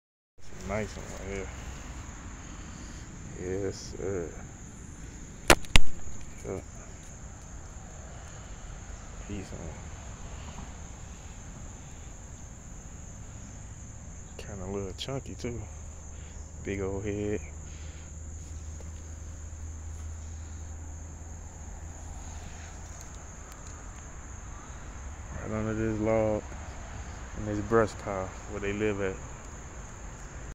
Shovel Head
Another creek adventure catching game fish from tiny holes in small creeks and relocating them to bigger, better environments so they can reach their full potential.